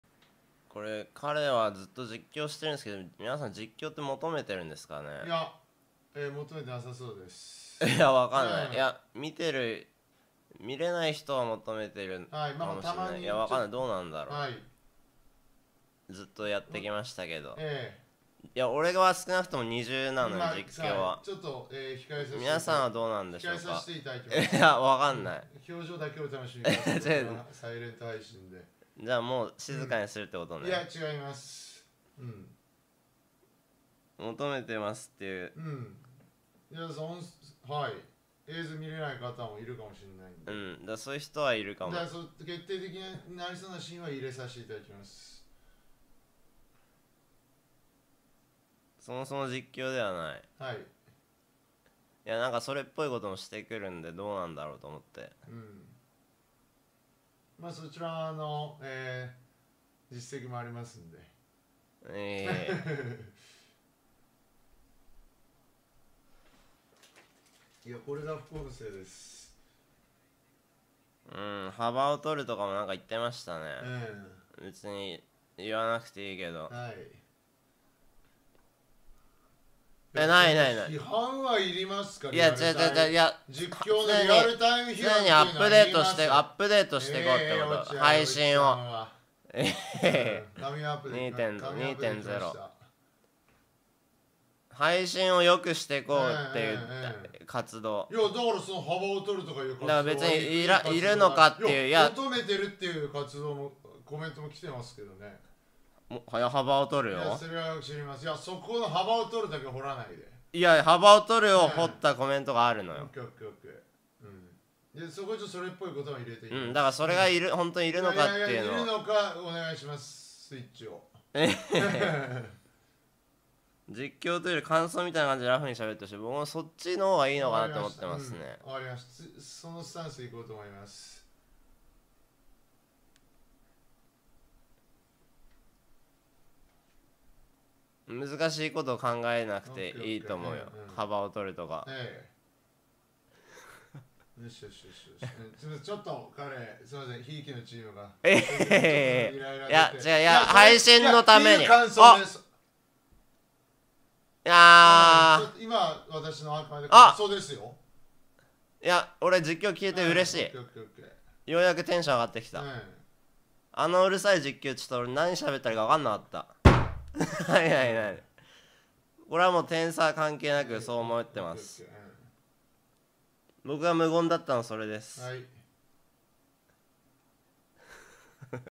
明け方から論争する漢達 (EURO2020決勝 イタリア対イングランド 実況)【コハロン切り抜き】
・KOHALON TOKYOさんのYoutubeチャンネル

コハロンとぽんPは私達を笑顔にさせる。